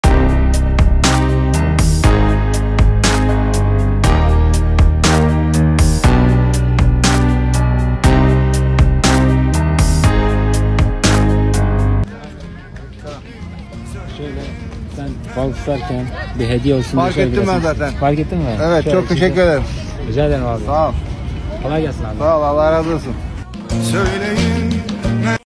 Balık Tutan Amcayı Çizdim.Tepkisi Çok İyiydi !/Sokak Sanatçısı
merhabalar. sokakta gezerken denk gelen insanları o anda çizip hediye ediyorum.Sıradaki sen olabilirsin :)